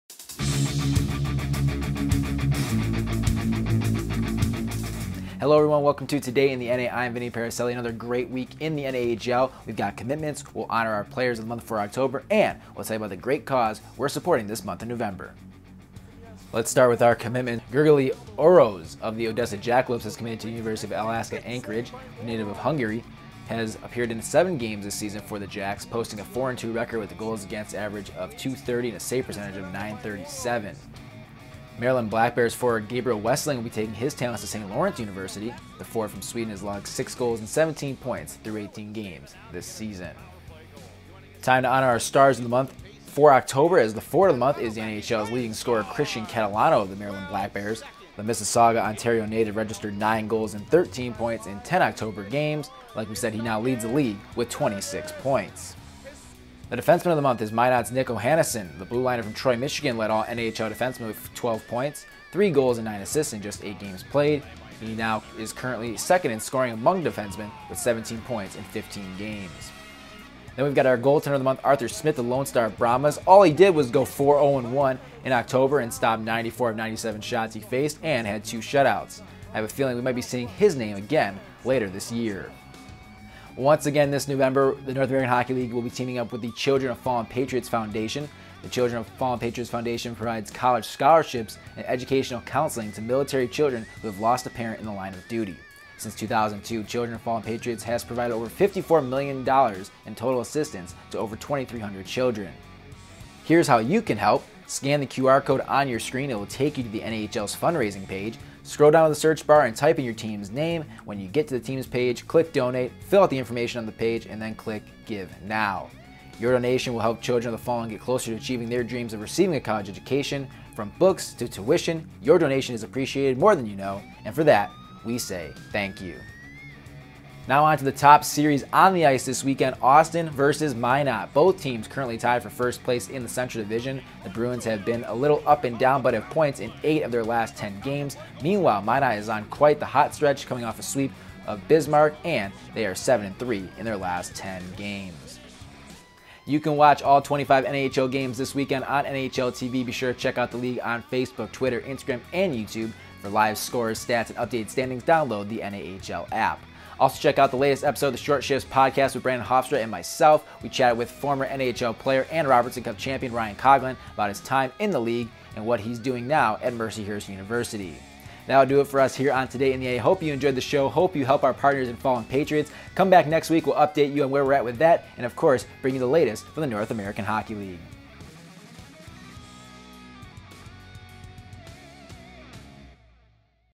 Today in the NA - 11/4/22
This week on Today in the NA, we highlight our players of the month for October and the latest NCAA commits. We also check out the NAHL's November fundraiser and a series between the top teams in the Central Division